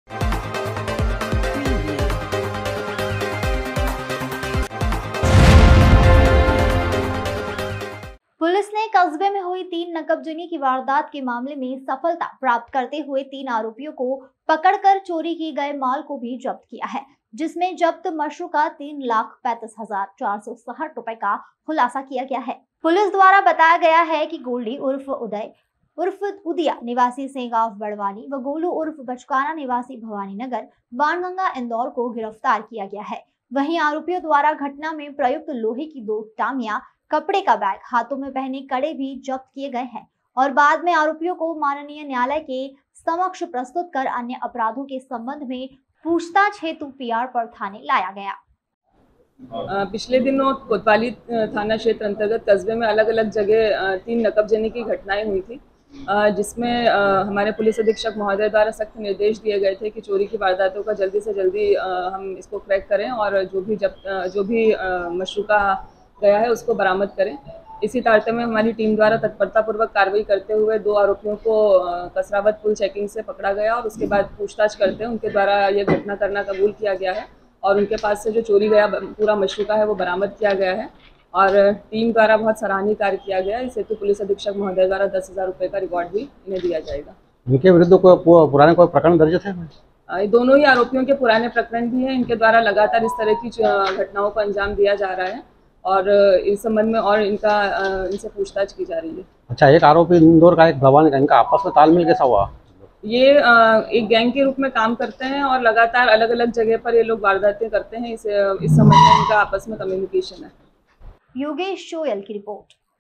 ESN-तीन नकबजनी की वारदात के मामले में बड़ी सफलता लाखो का माल....
तीन नकबजनी की वारदात के मामले में सफलता प्राप्त